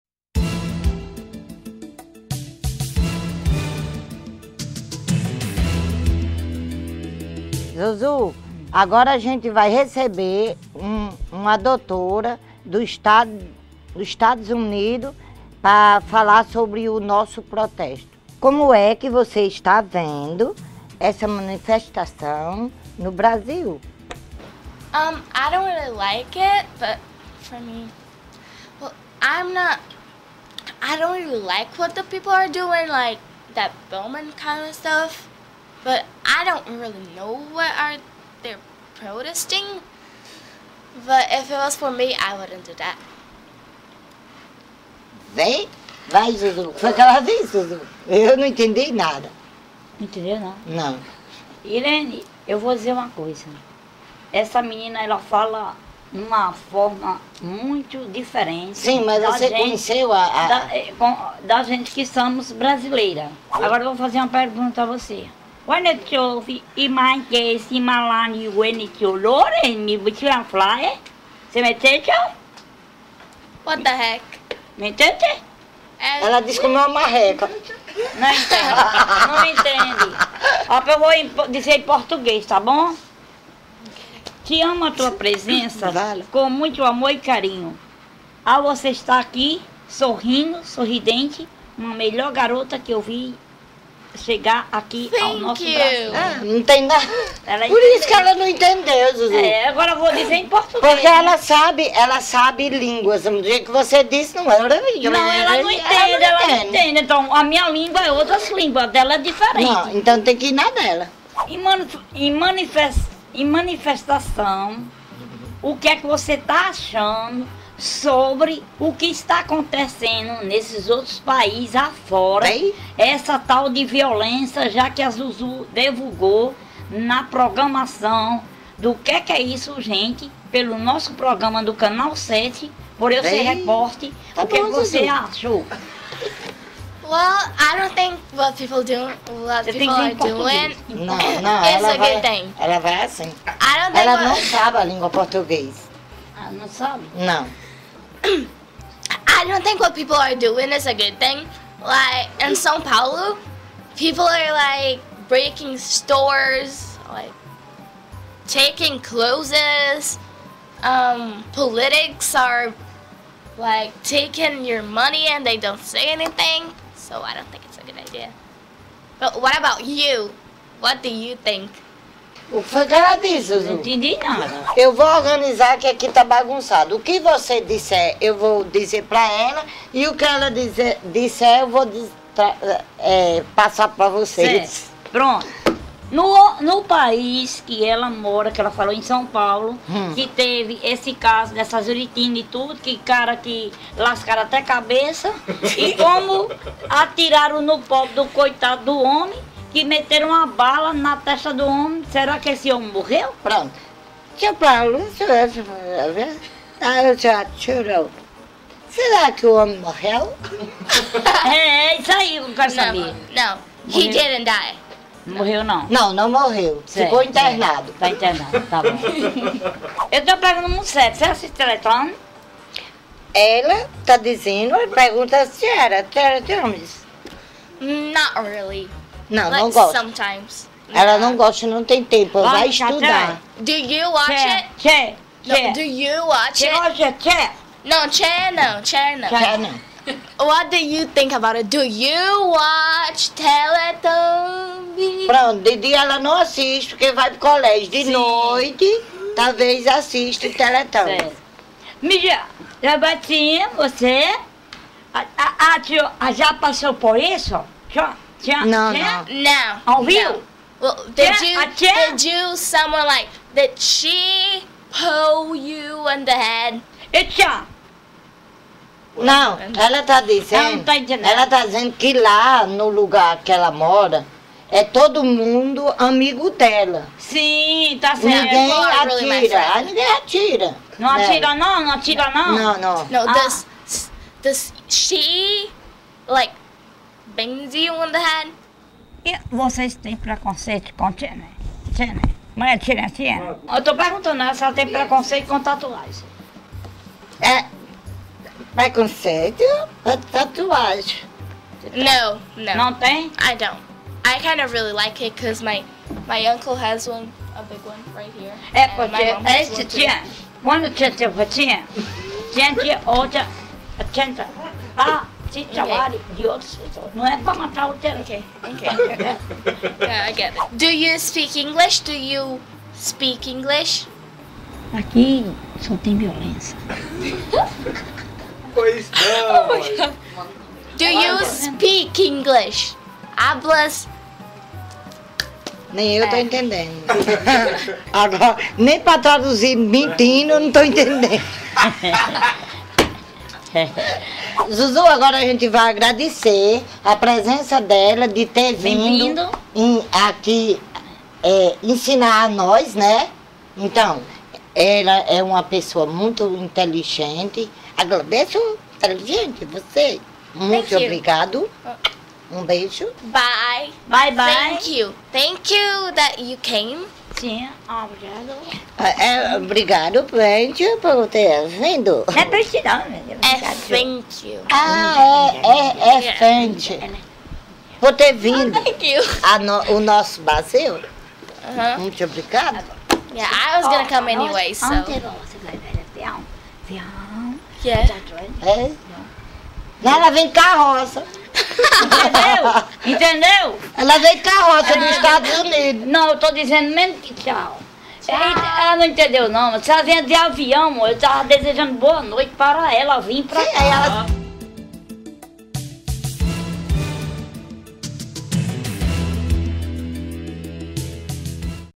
ZUZU E DONA IRENE CONVERSAM COM ESTRANGEIRA SOBRE PROTESTOS
Produção: Jedson Leandro / Keké Oliveira
Imagens: Jedson Leandro
Edição: Jedson Leandro